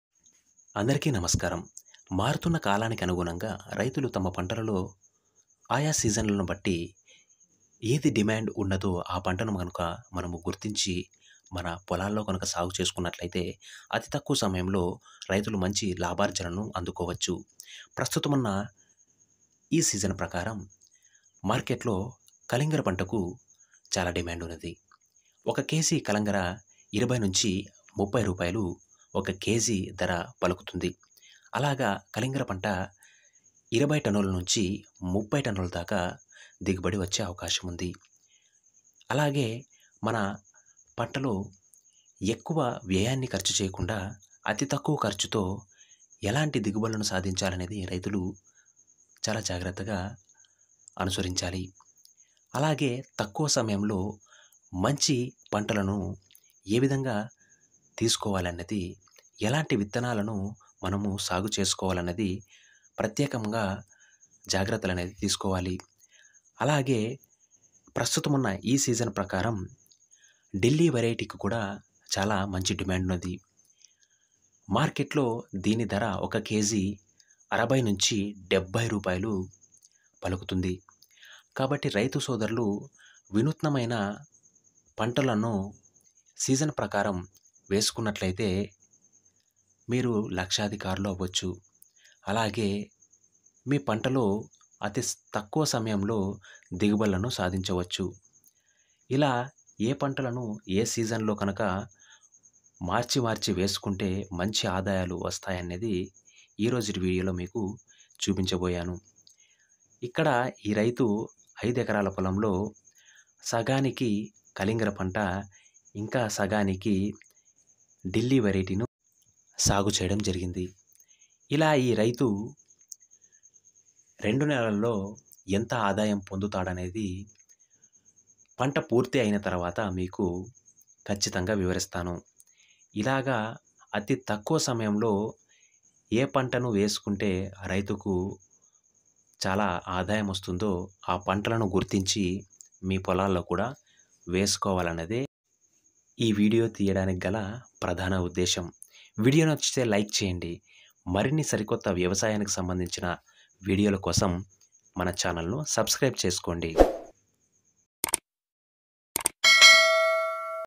సిరులు కురిపిస్తున్న కలింగర పంట కేజీ ధర మార్కెట్లో 30రూపాయలు,కాని ఏ సీజన్లో పెట్టుకోవాలిwatermilon
సిరులు కురిపిస్తున్న కలింగర పంట కేజీ ధర మార్కెట్లో 30రూపాయలు,కాని ఏ సీజన్లో పెట్టుకోవాలి/High dimond on watermelon crop